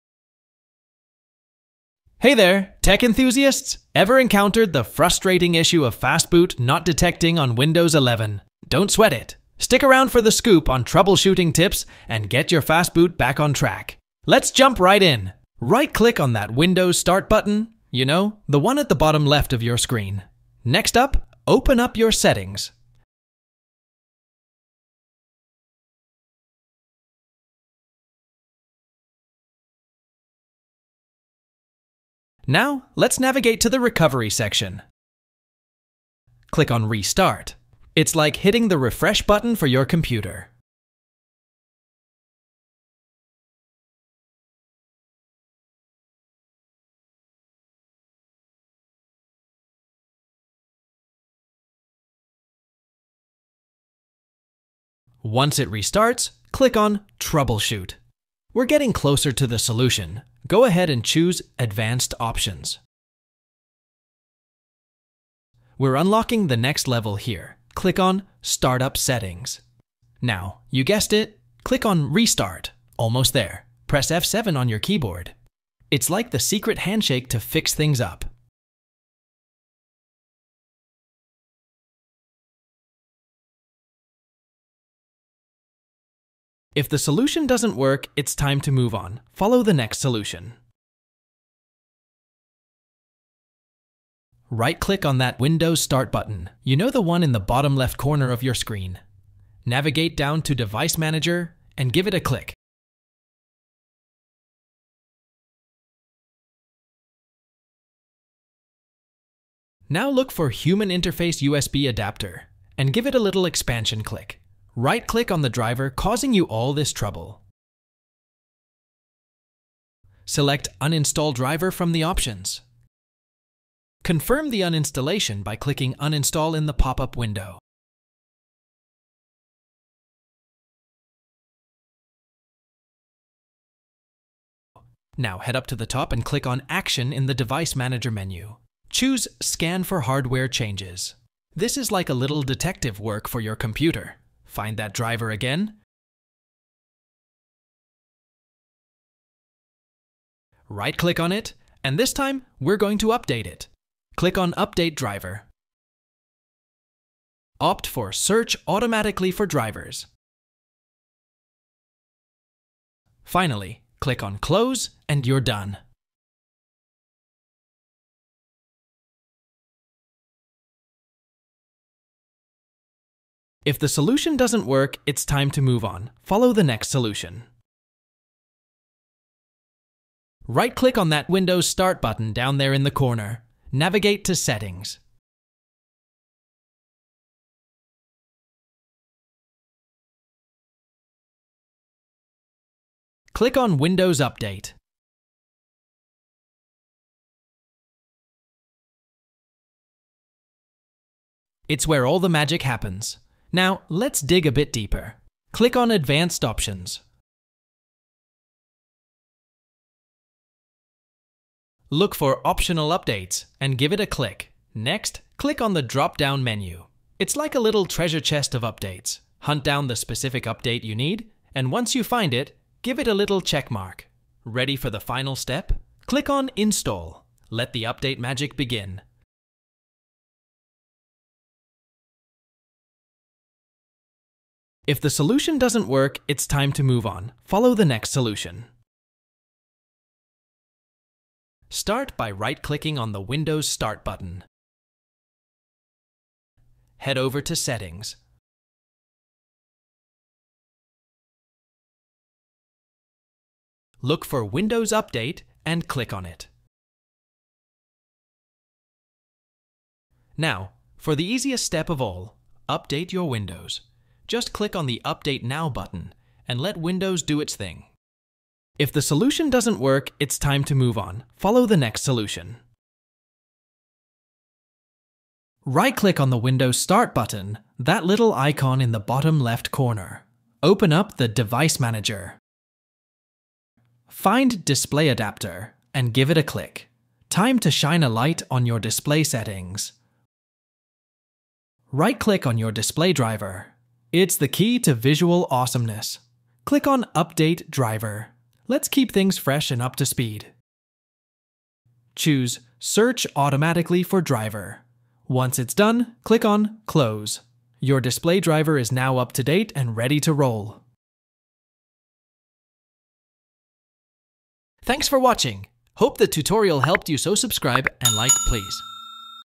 How To Fix Fastboot Not Detecting on Windows 11/10 Problem
Hey there, tech enthusiasts! Ever encountered the frustrating issue of Fastboot not detecting on Windows 11? Don't sweat it! Stick around for the scoop on troubleshooting tips and get your Fastboot back on track. Let's jump right in!

🔧 Having trouble with Fastboot not detecting on Windows 11? Fret not! 🚀 In this step-by-step guide, we'll walk you through the quick and easy solutions to get your Fastboot back in action. From troubleshooting tips to hands-on fixes, we've got you covered. Say goodbye to the hassle and hello to smooth sailing with your Windows 11 Fastboot. Dive into the video now and empower your tech game! 💻✨

00:00 Intro
00:14 Disable driver signature enforcement
01:33 Human interface usb reinstall
Update driver
03:00  Optional Updates
04:01 Update windows
04:35 Reinstall display driver
05:14 Outro